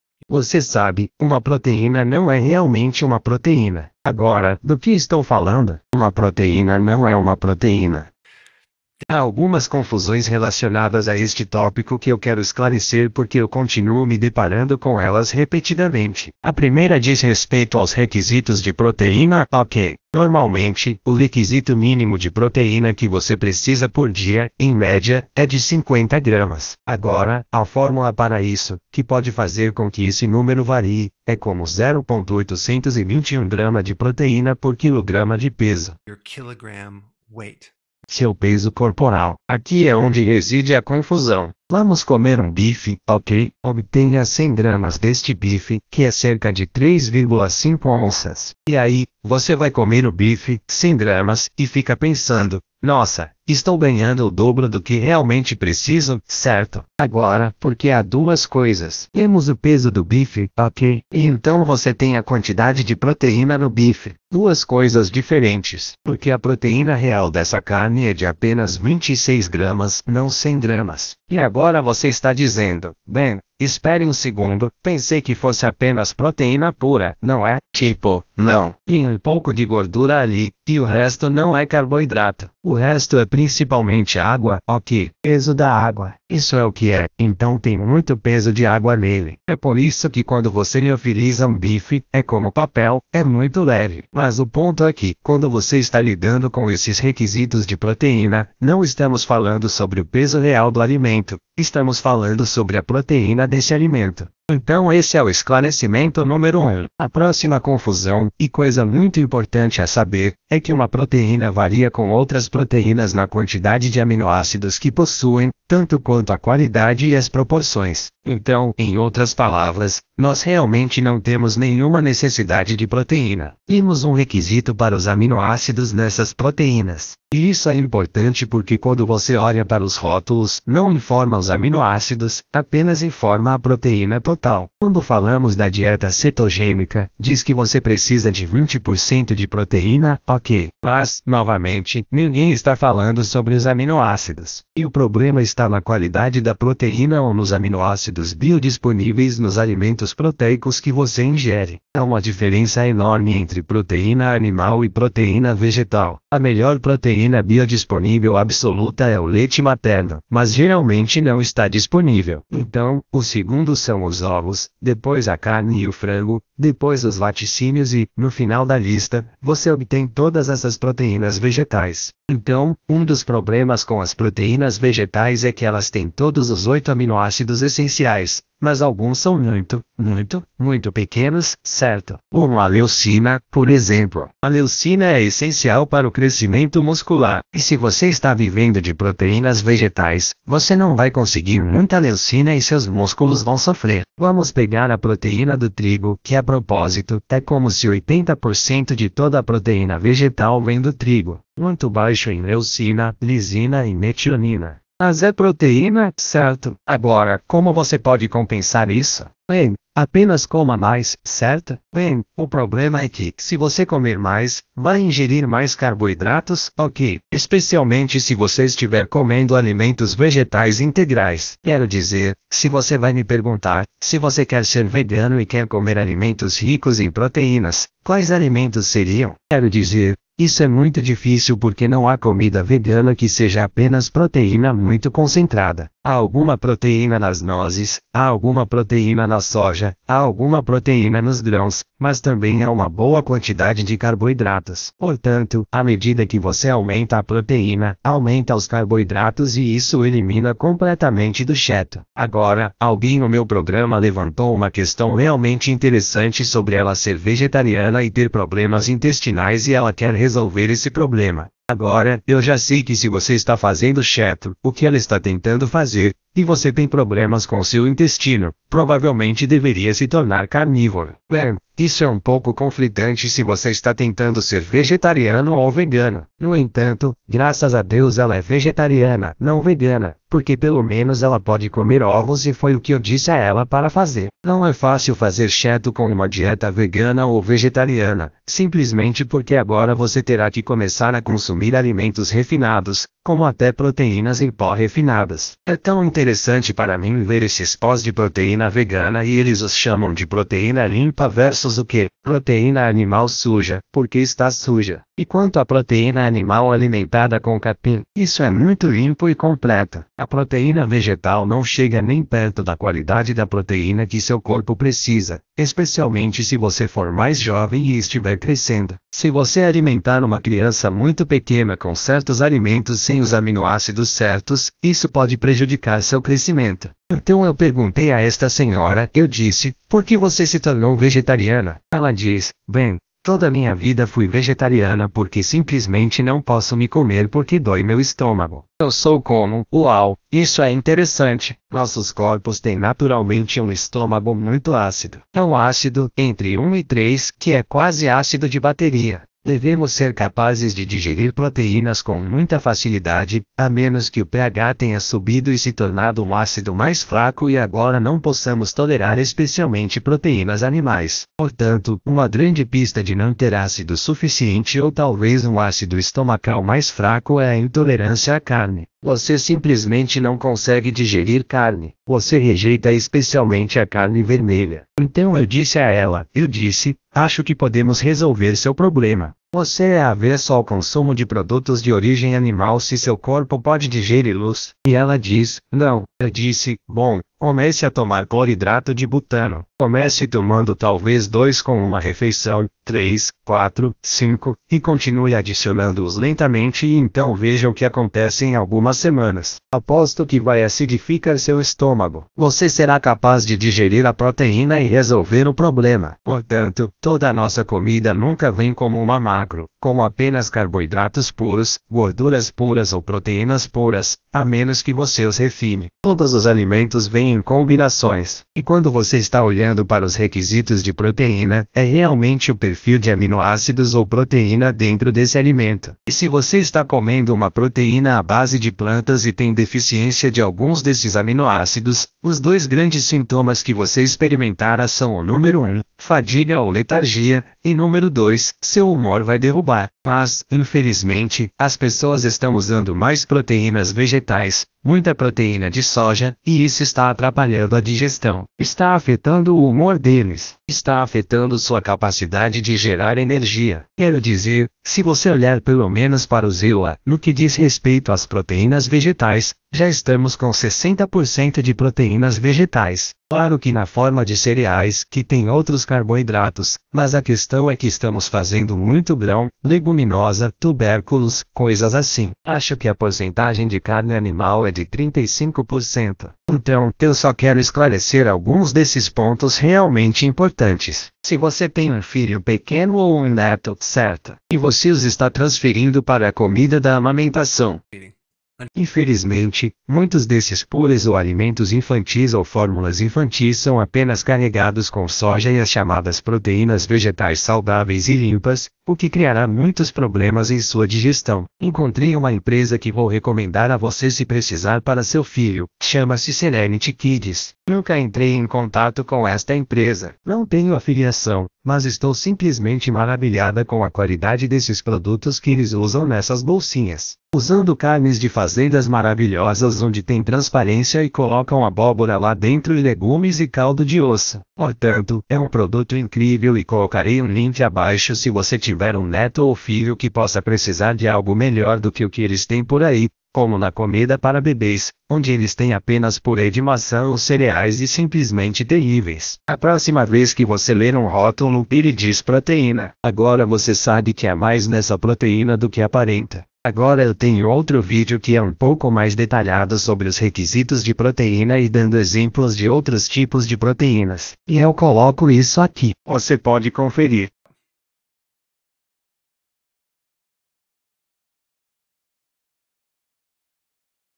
Proteína não é apenas proteína
Proteína não é apenas proteína. Existem diferenças na qualidade e aminoácidos entre as diversas fontes de proteínas, tanto animais quanto vegetais. Isso pode afetar a absorção e utilização pelo corpo, especialmente quando se segue uma dieta cetogênica ou vegetariana/vegana.

Fatos
💪 A quantidade de proteína necessária diariamente varia de acordo com o peso corporal e não com o peso da comida ingerida.
💪 Nem todas as proteínas têm a mesma qualidade ou proporção de aminoácidos essenciais.
💪 Alimentos vegetais têm proteínas, mas nem sempre são um substituto completo para as fontes animais.
💪 Alguns alimentos com proteínas vegetais têm pouco ou nenhum aminoácido essencial para a construção muscular adequada.
💪 Alimentação rica em proteínas vegetais pode conter muitos carboidratos, o que pode expulsar a chance de uma dieta cetogênica.
💪 Acidificar o estômago com HCL pode ajudar o corpo a digerir proteínas animais para quem as encontra difíceis de tolerar.
💪 Como nossos corpos naturalmente têm um estômago muito ácido, é normal ser capaz de digerir proteínas animais com facilidade.